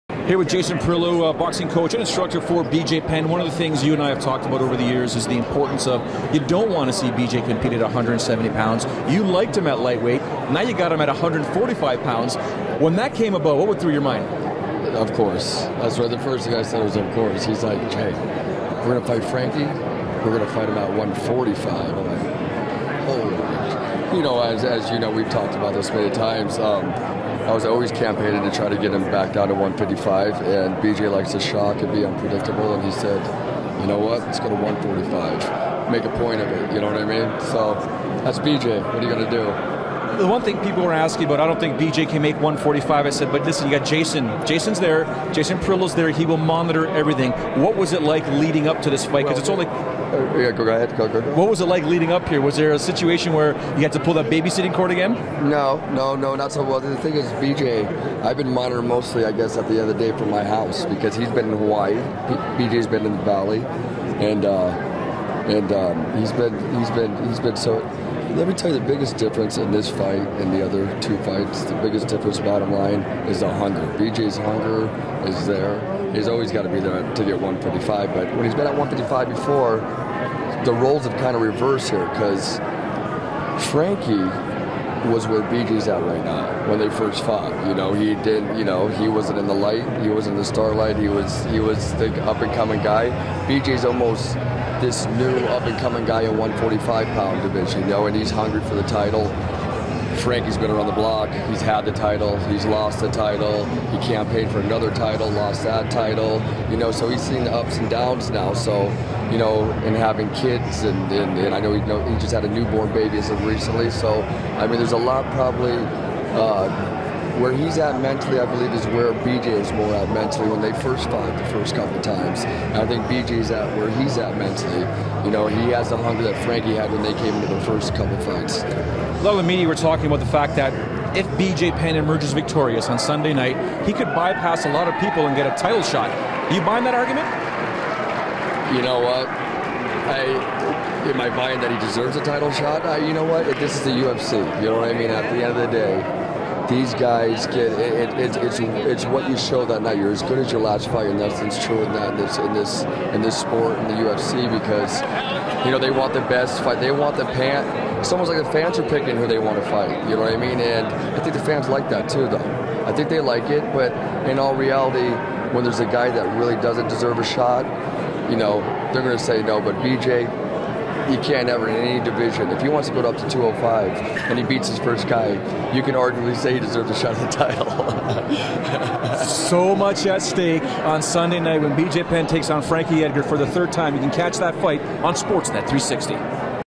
Parillo keeping Penne a hungry fighter
Jason Parillo is coaching BJ Penn, making sure he keeps him level-headed considering Penn's tendency to go for shocking moves like a significant weight loss.